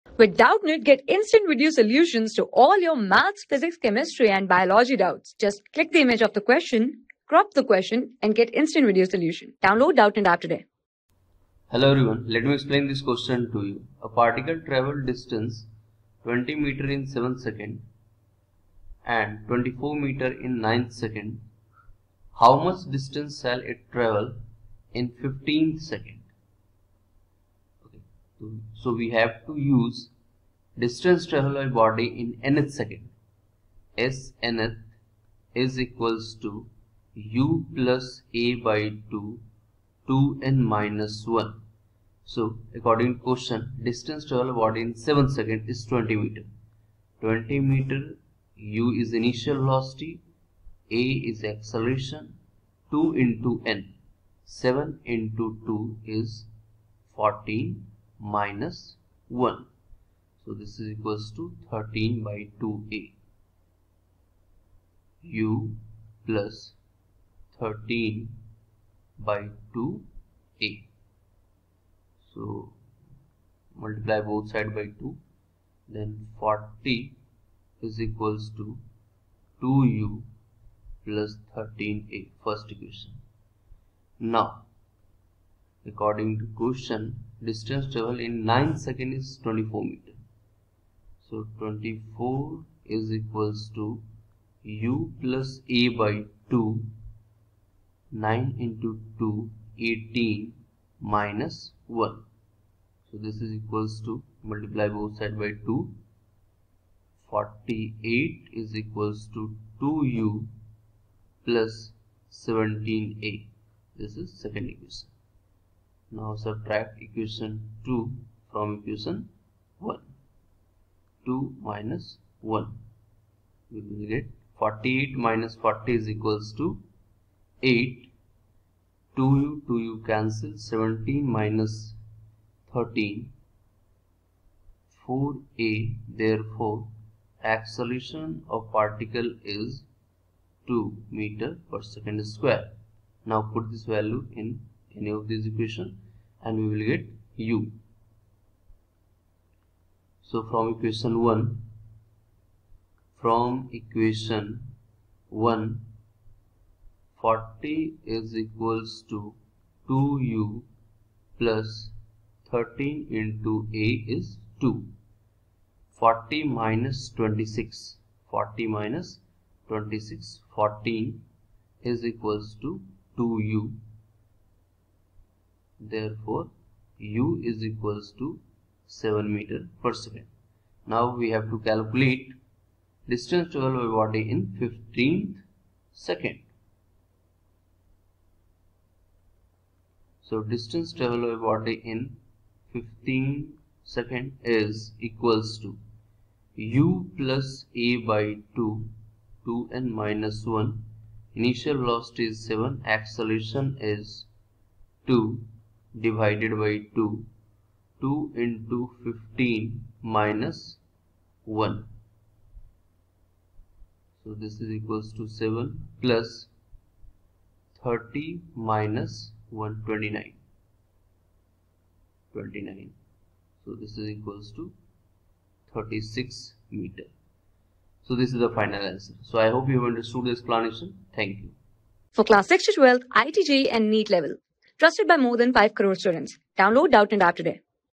A particle travels a distance of 20 m in the 7^(th) seconds and 24 m in 9^(th) second. How much ...
A particle travels a distance of 20 m in the 7^(th) seconds and 24 m in 9^(th) second. How much distance shall it travel in the 15^(th) second?
Class: 12
Subject: PHYSICS
Chapter: MOTION IN STRAIGHT LINE
Board:IIT JEE

👉 Have Any Query? Ask Us.